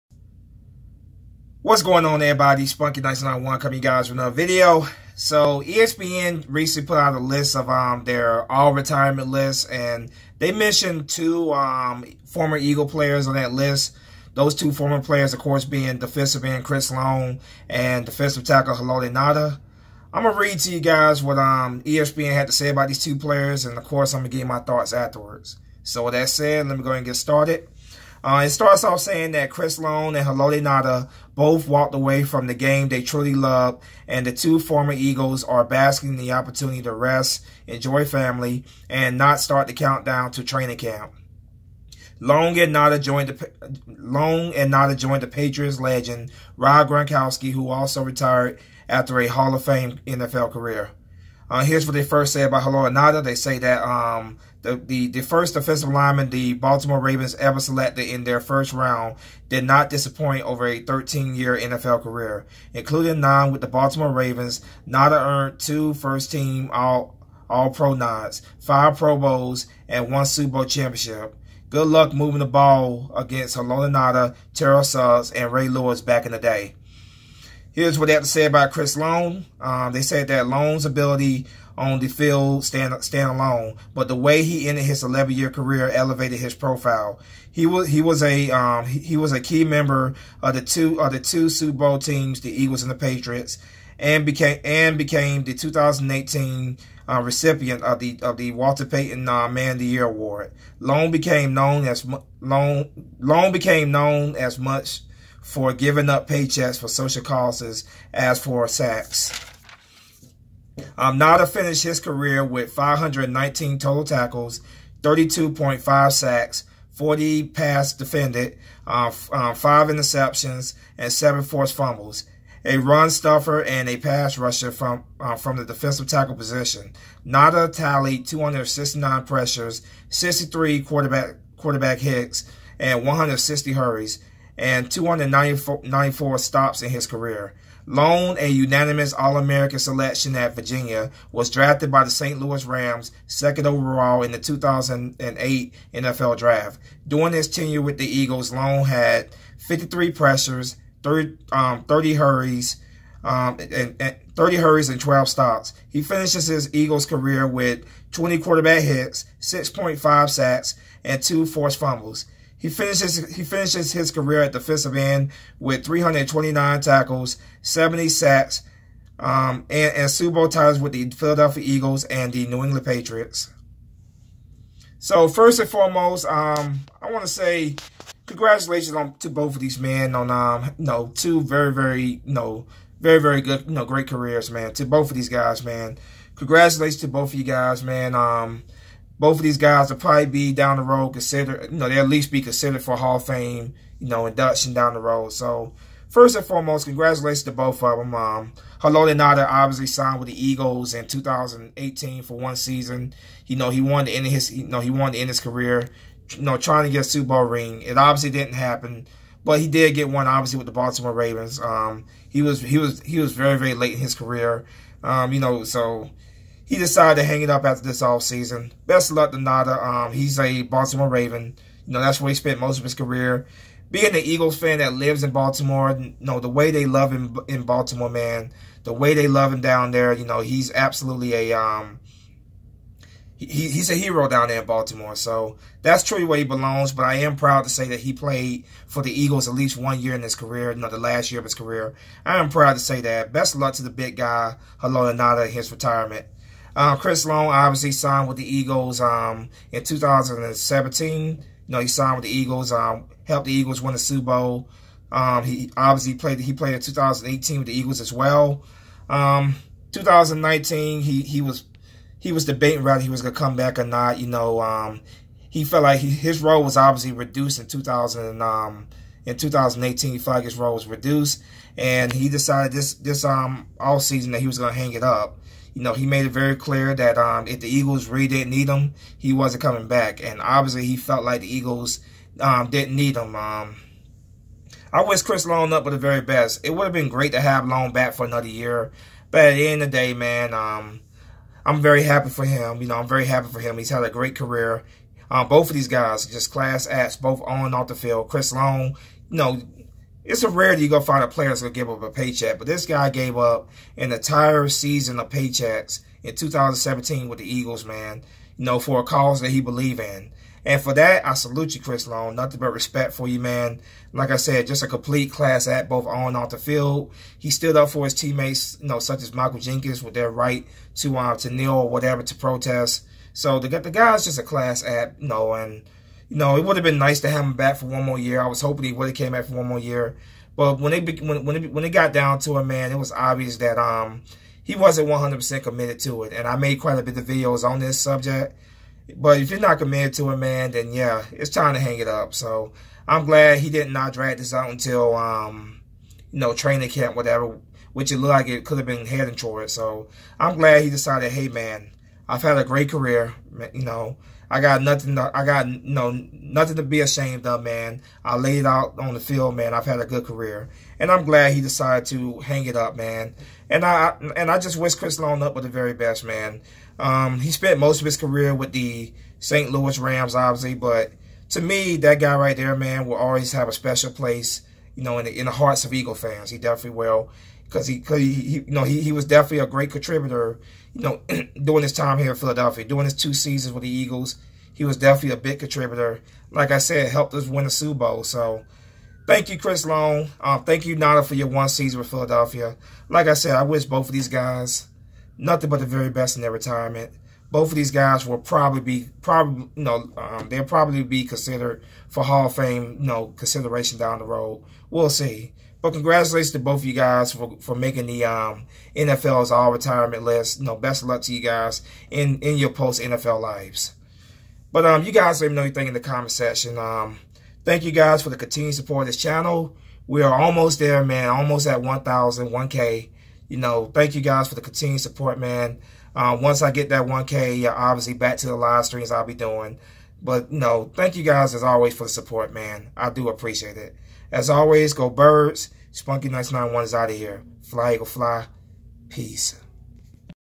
Chris Long/Haloti Ngata named to ESPN's All-Retirement list
Former Eagles Chris Long and Haloti Ngata named to ESPN's All-Retirement list.